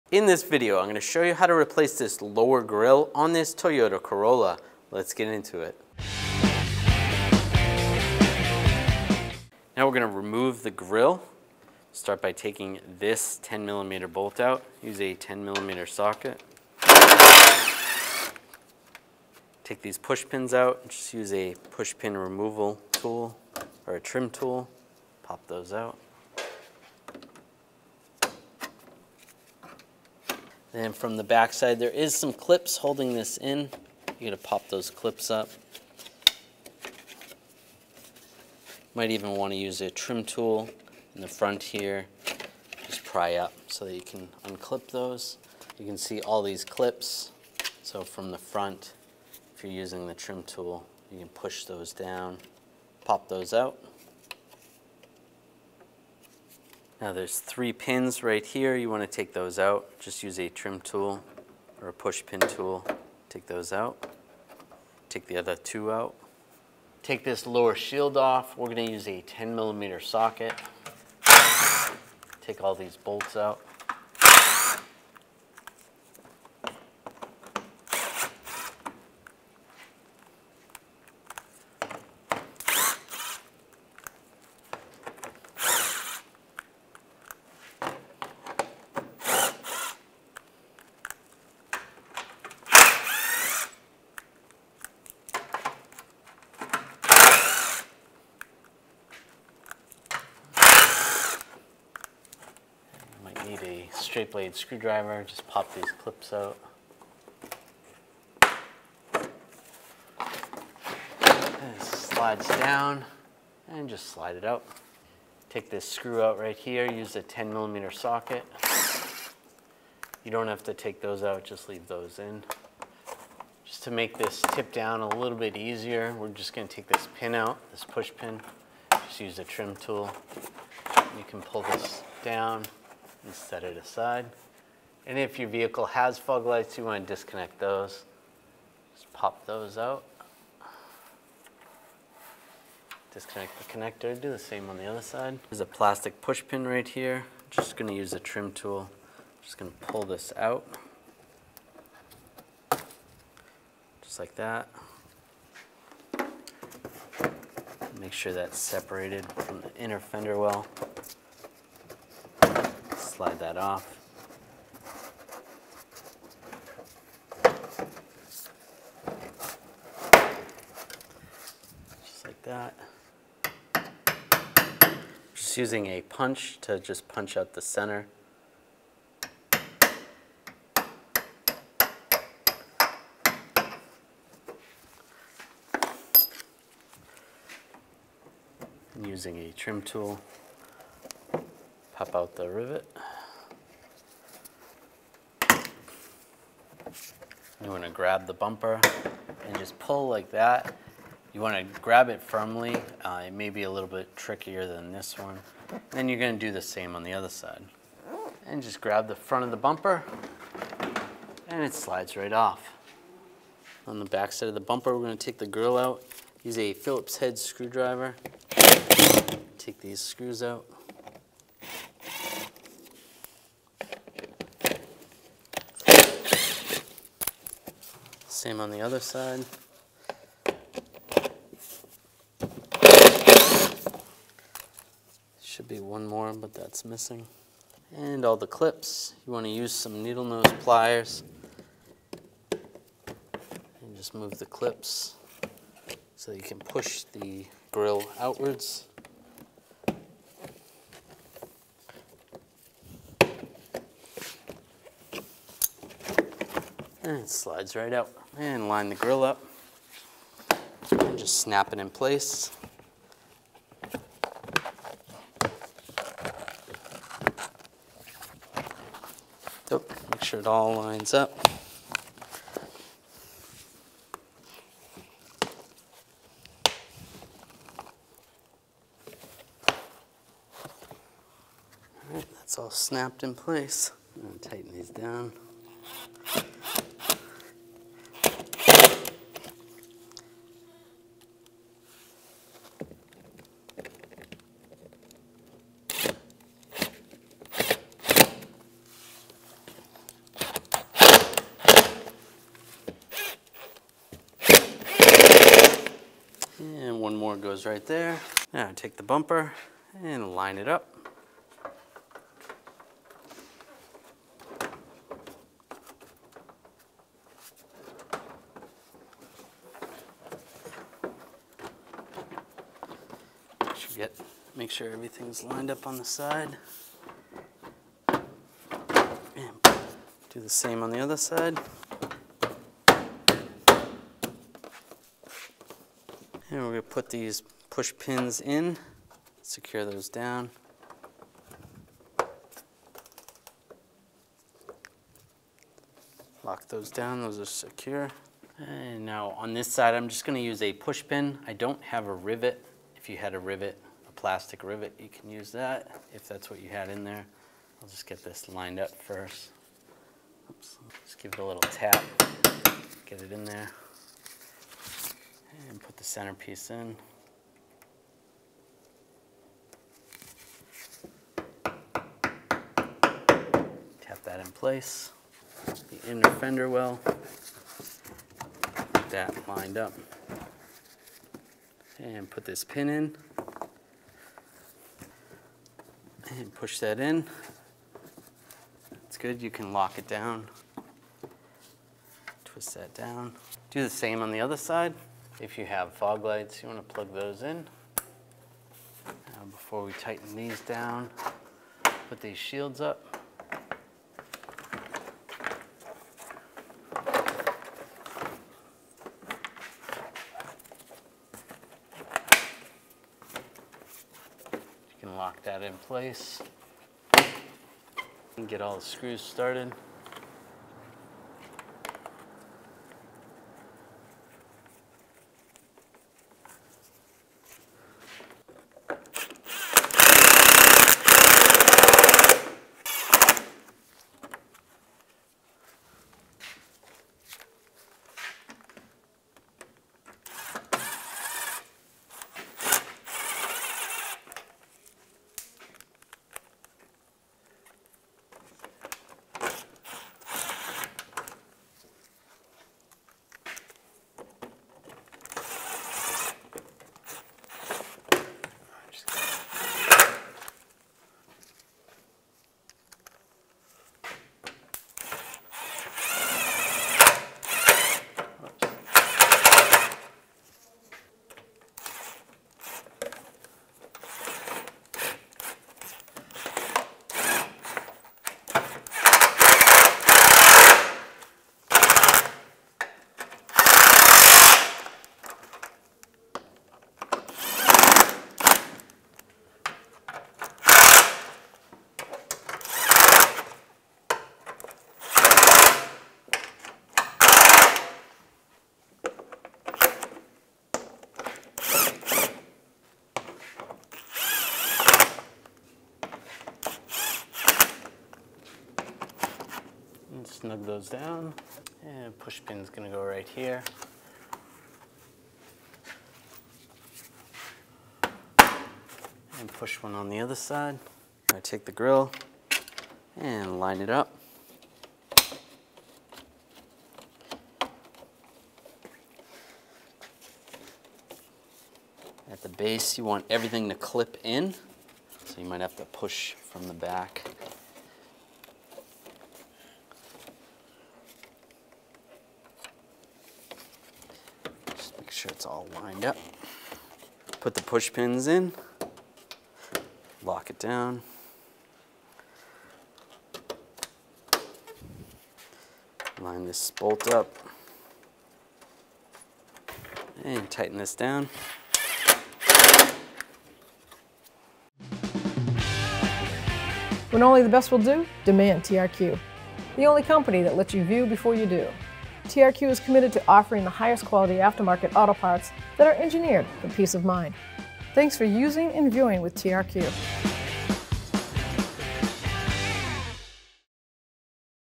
How to Replace Bumper Mounted Grille 2014-2019 Toyota Corolla
This video shows you how to install a bumper mounted grille on your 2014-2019 Toyota Corolla. Grilles on today’s vehicles are for the most part plastic. And, they are often the first point of contact between your vehicle and rocks, debris, or worse. Because of this, they crack or break easily, and they do actually protect sensitive parts like radiators and air conditioning condensers.

This process should be similar on the following vehicles:
2014 Toyota Corolla
2015 Toyota Corolla
2016 Toyota Corolla
2017 Toyota Corolla
2018 Toyota Corolla
2019 Toyota Corolla

• Punch